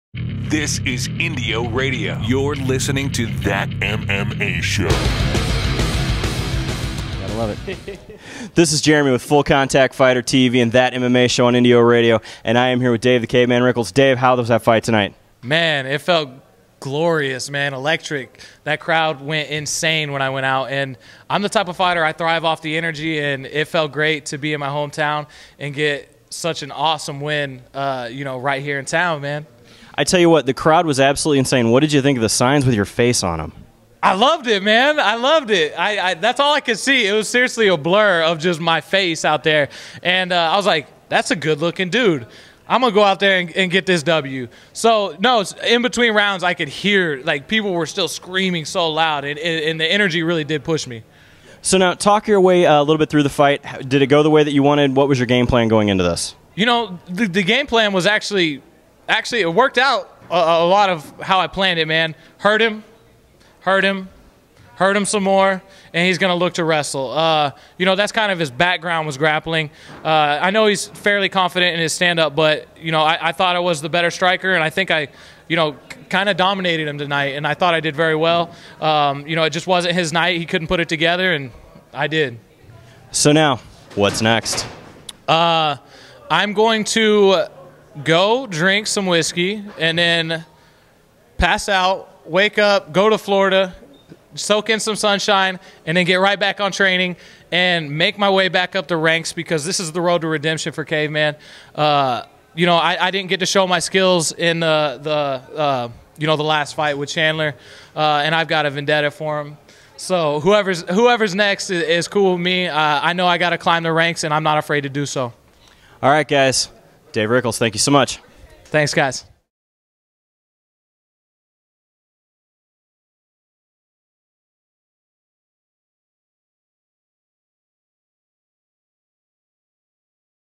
That MMA show speaks with Dave "The Cave Man" Rickels after his win
We sit down with The Caveman after his commanding win at Bellator 103 over JJ Ambrose. We discuss what happens next and how the crowd affected his fight.